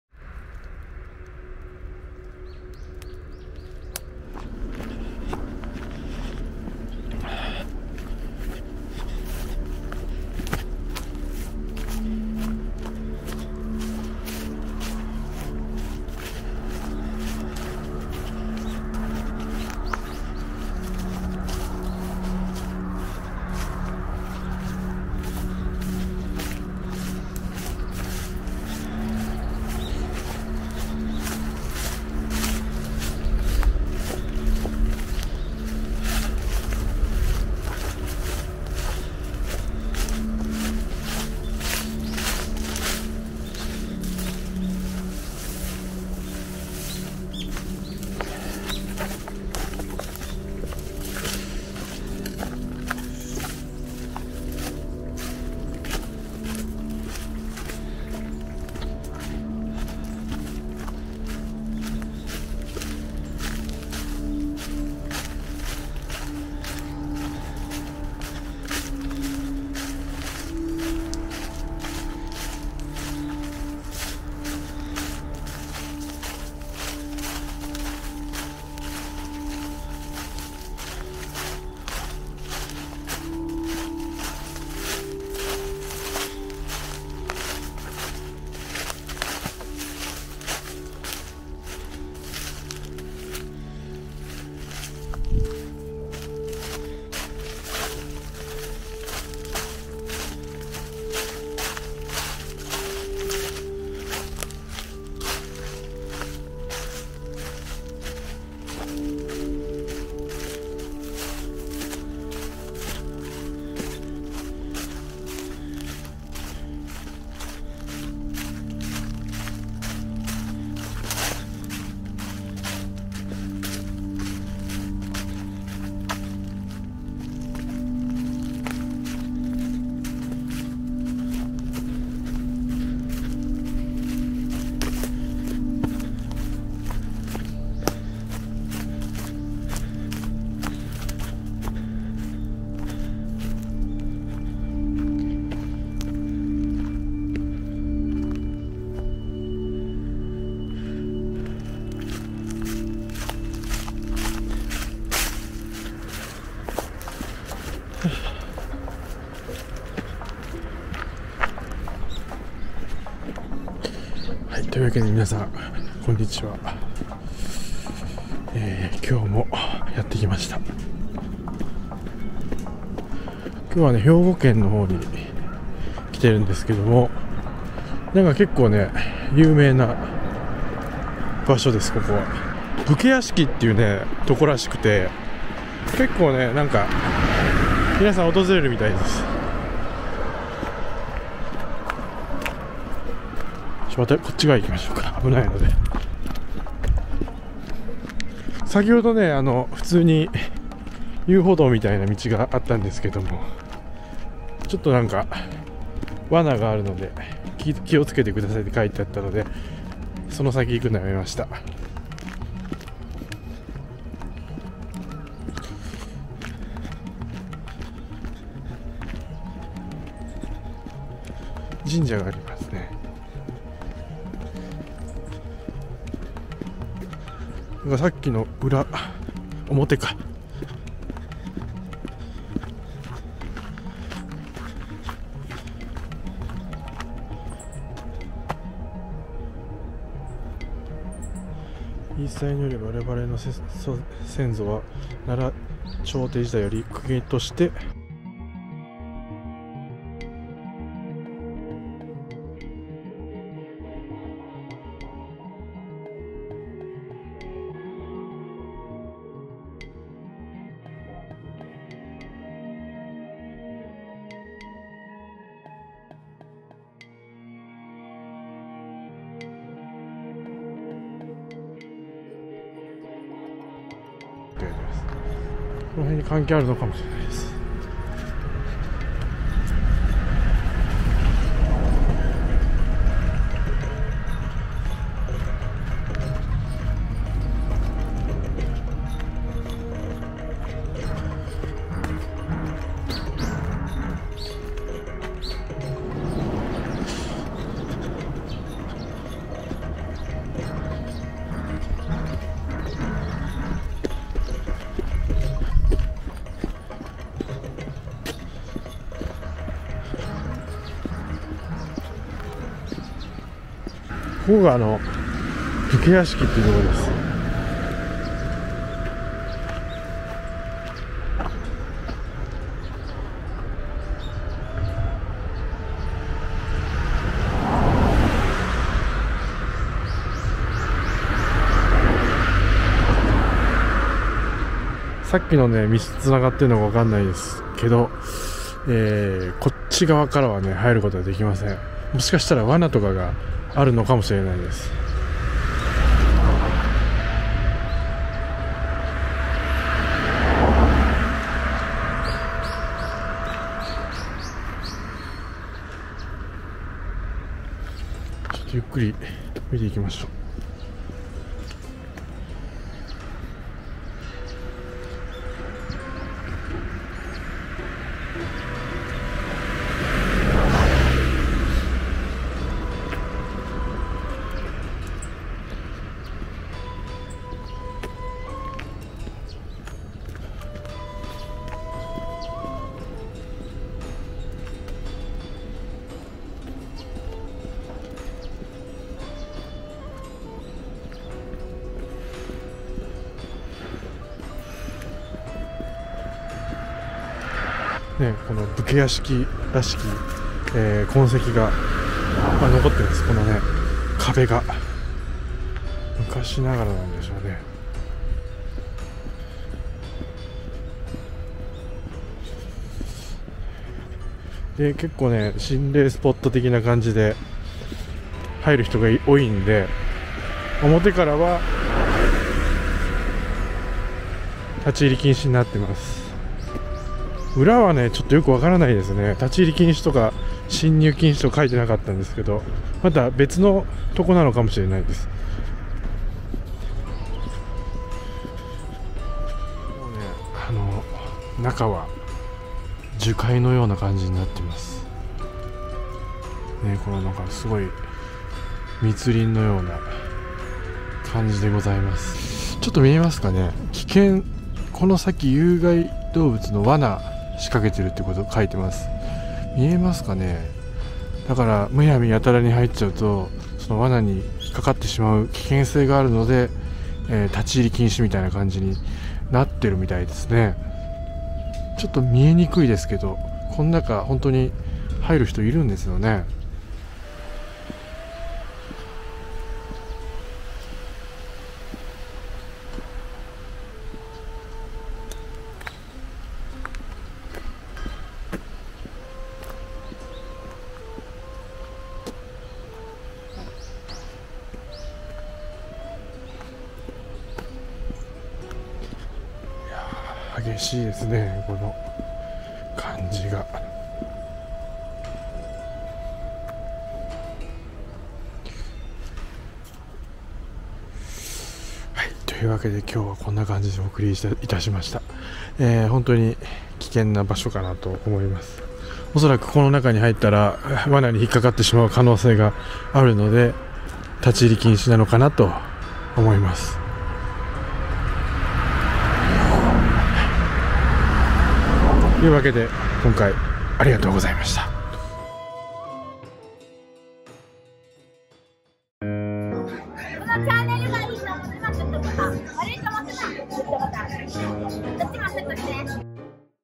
兵庫県加古川市【恐怖の心霊スポットとして恐れられた武家屋敷】武家の女人の霊を度々目撃するらしい《立入禁止の理由》
参考

再生リスト